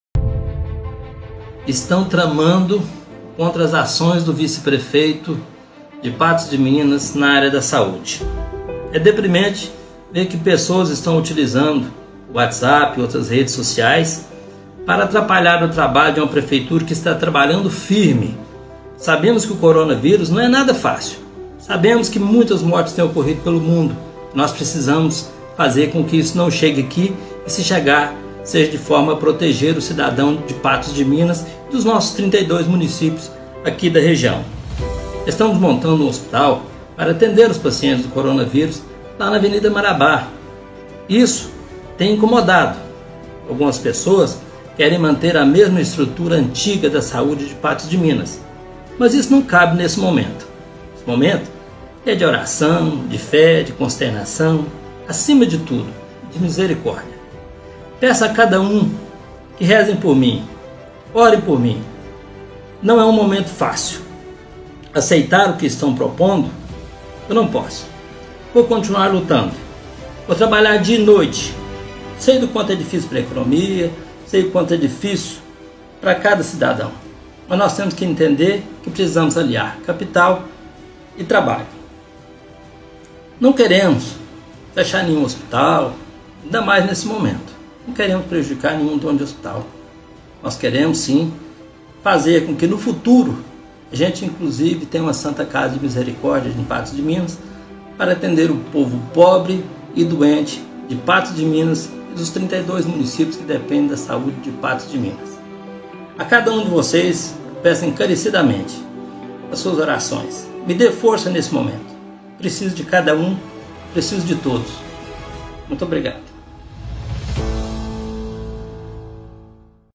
Paulo Mota diz que é alvo de tramóias
Segundo ele, grupos influentes estariam prejudicando as ações dele em prol da saúde na cidade.